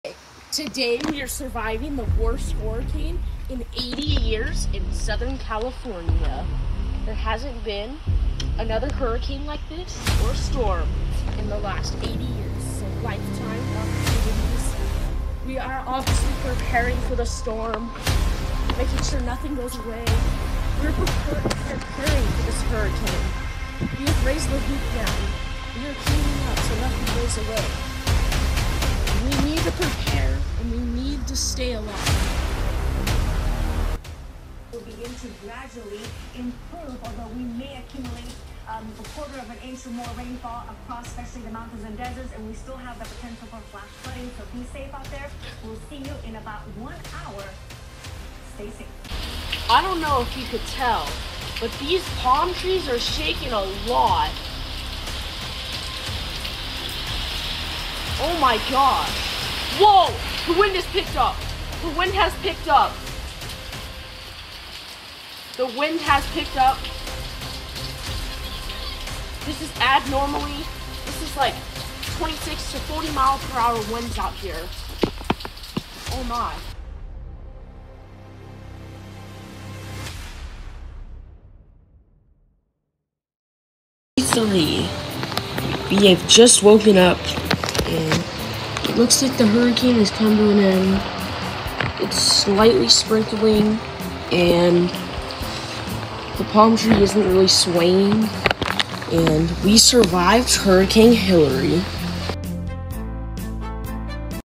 Surviving Hurricane Hilary!
Today we survive a category 4 hurricane! The worst storm since the last 80 years!